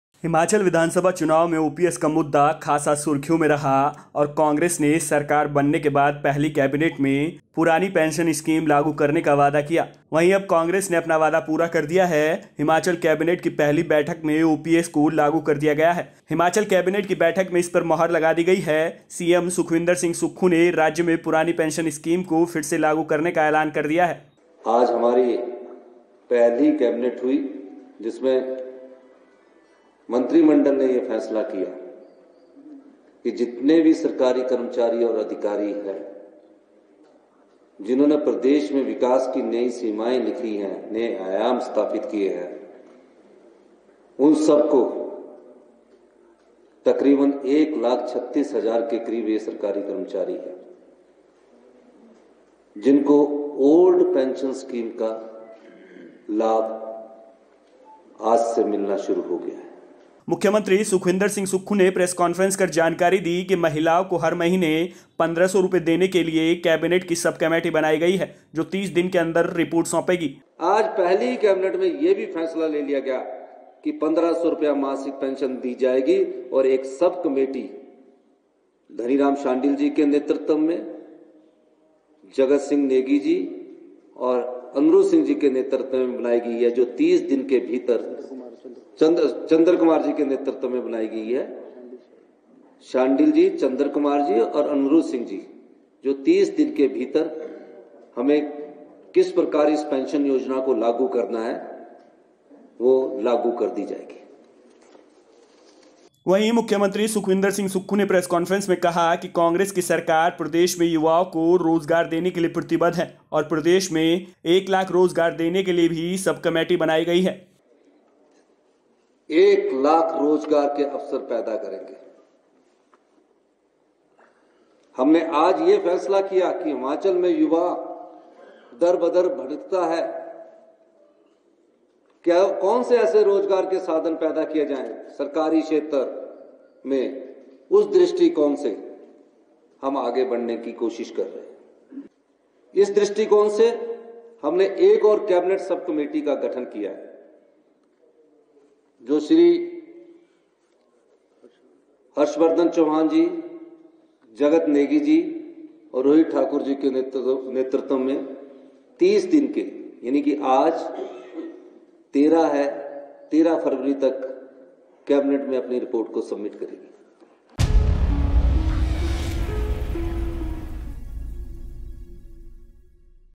हिमाचल में पुरानी पेंशन स्कीम फिर लागू, मुख्यमंत्री सुखविंदर सिंह सुक्खू ने कैबिनेट में लगाई मुहर
1.हिमाचल में पुरानी पेंशन स्कीम फिर लागू, मुख्यमंत्री सुखविंदर सिंह सुक्खू ने कैबिनेट में लगाई मुहर
2.हिमाचल कैबिनेट की पहली बैठक में OPS को लागू कर दिया गया है...हिमाचल कैबिनेट की बैठक में इस पर मुहर लगा दी गई है...सीएम सुक्खू ने राज्य में पुरानी पेंशन स्कीम को फिर से लागू करने का ऐलान कर दिया है...